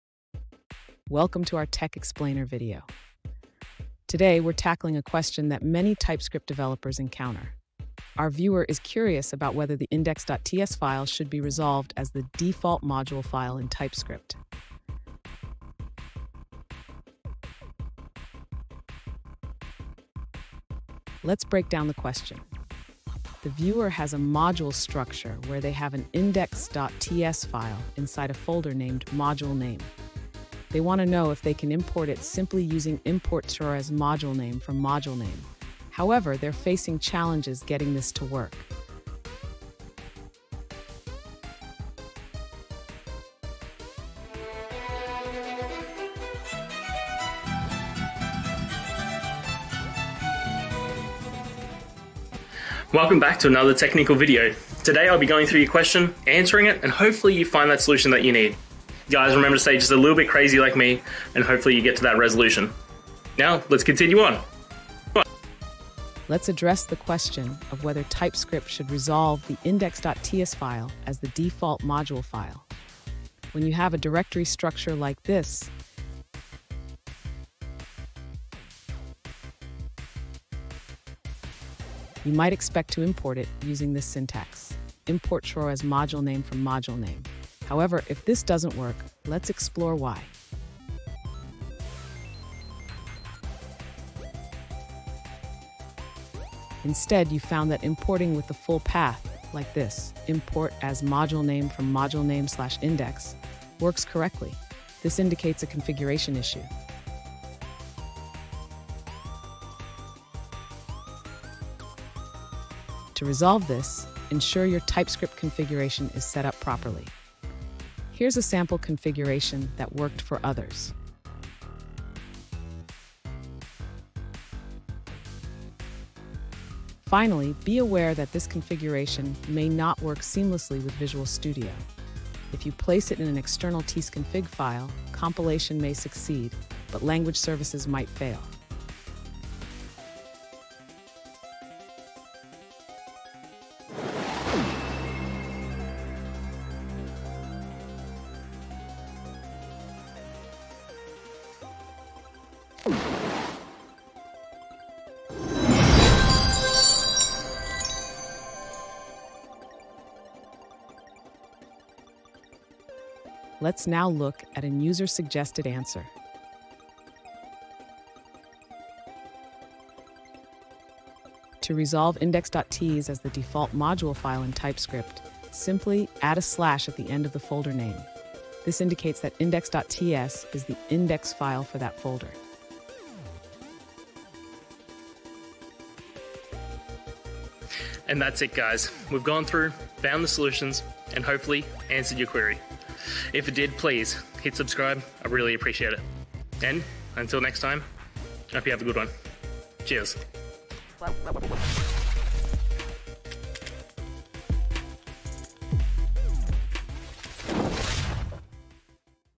Should TypeScript Resolve index.ts as Default Module File? Pros and Cons
In this video, we delve into the ongoing debate surrounding TypeScript's handling of default module resolution, specifically whether it should automatically recognize `index.ts` as the default module file. As TypeScript continues to evolve, understanding the implications of this feature is crucial for developers. Join us as we explore the pros and cons of this approach, examining how it can impact project organization, developer experience, and code maintainability.

Today's Topic: Should TypeScript Resolve index.ts as Default Module File? Pros and Cons

Related to